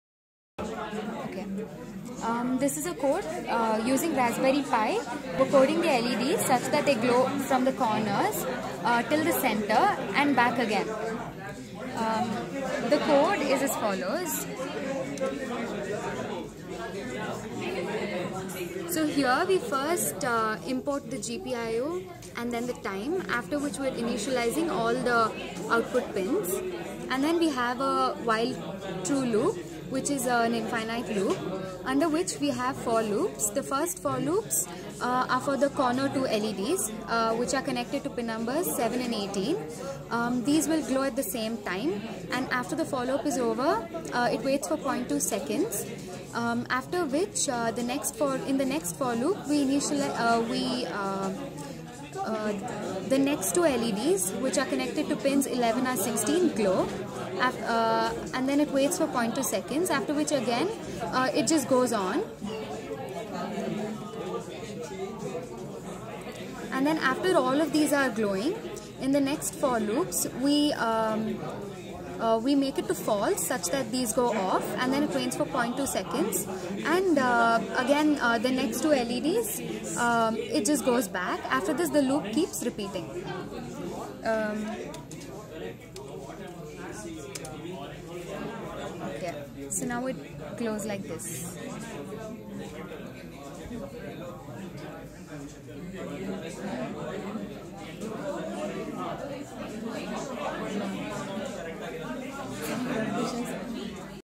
Raspberry pi MODEL B+ LED BLINKING using python language
Led blinking pattern  using raspberry pi .using python language. MODEL B+ 40 PINS